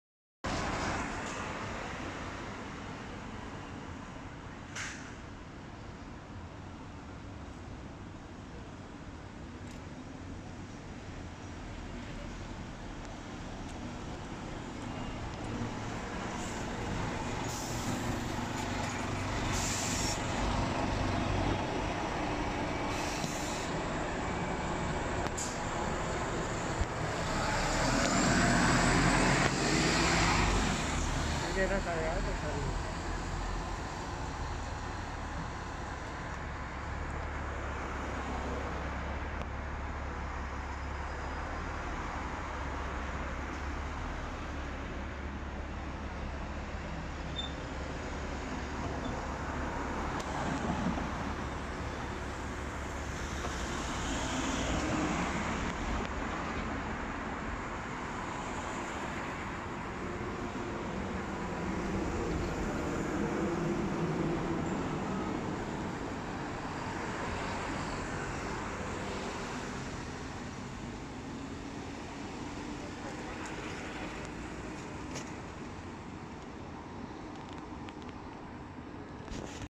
[2X] SBST Svc 90 Scania K230UB Euro 5
The Scania K230UB Gemilang Coachwork bus is the most common bus in Singapore. The Bodywork look very similar to Kuala Lumpur, Melbourne and other countries.

Video was filmed on: 18 July 2021